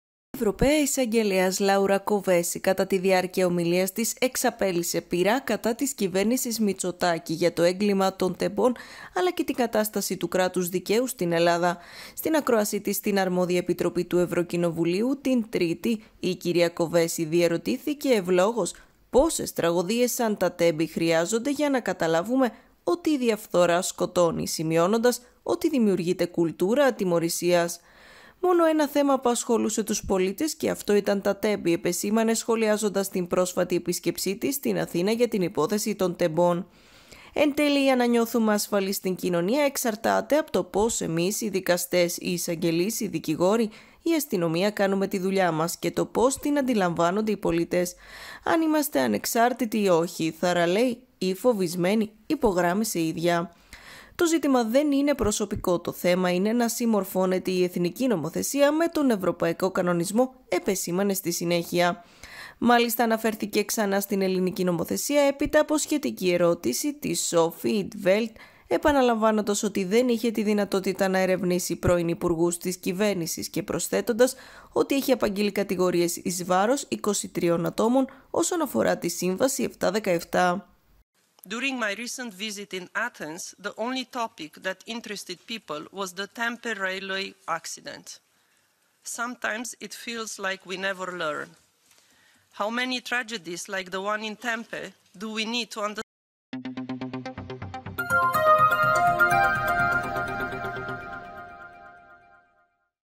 Σφοδρά «πυρά» της Ευρωπαίας Εισαγγελέως κατά της κυβέρνησης Μητσοτάκη για την σφαγή των Τεμπών
Σφοδρά «πυρά» της Ευρωπαίας Εισαγγελέως κατά της κυβέρνησης Μητσοτάκη για την σφαγή των Τεμπών  | Pronews TV